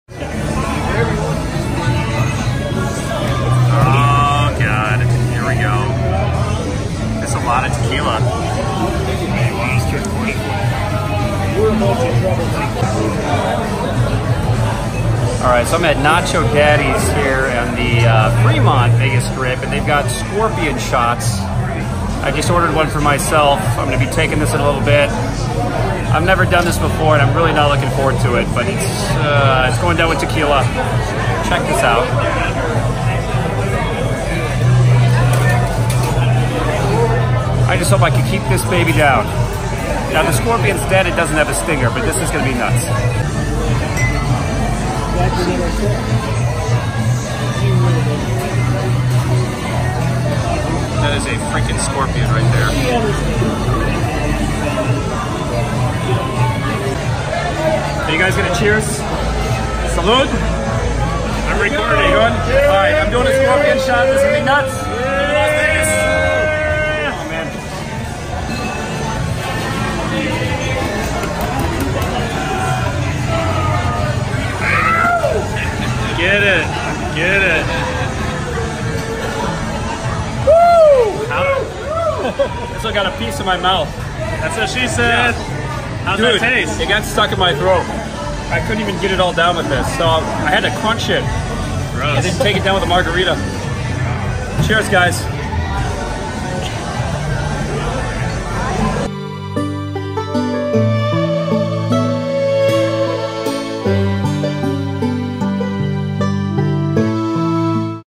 Taking a Scorpion Shot In Las Vegas
A visit to Las Vegas is not complete without taking a scorpion shot with tequila at Nacho Daddy, that's right a dead scorpion floating in a shooter glass of tequila. I'm not one to turn a challenge down, glad I got to keep the shooter shot glass. For those curious, a scorpion is rough texture going down - be prepared to crunch on it.